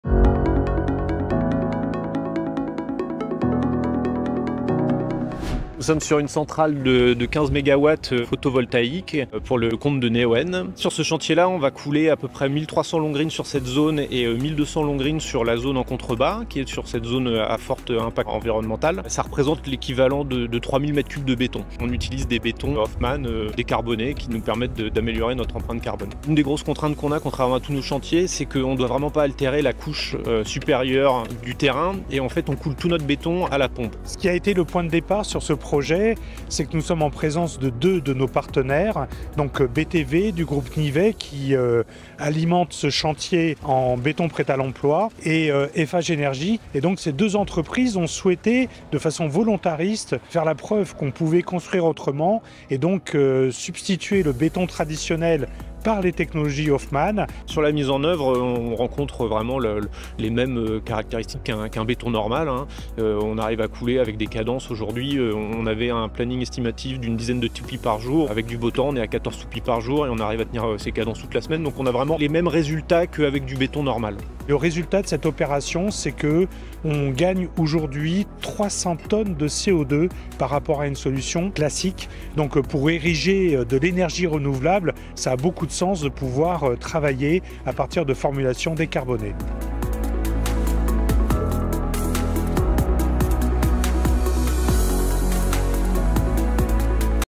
🚧 I CHANTIER I béton décarboné & énergie renouvelable - Eiffage
À l'initiative de nos deux partenaires, BTV Groupe Nivet et Eiffage Energie Systemes, notre technologie H-UKR, béton décarboné 0% clinker, a été utilisée pour la réalisation des fondations d'une centrale photovoltaïque dans le cadre de la valorisation d'un site pollué.

Avec un objectif commun de réduire au maximum l'empreinte carbone, sur ce chantier de Gétigné, en Région Pays de La Loire, plus de 3000 m3 de béton 0% clinker H-UKR ont été coulé pour la réalisation de 1200 longrines. Cela représente 300 tonnes de CO2 économisées par rapport à une solution béton conventionnelle.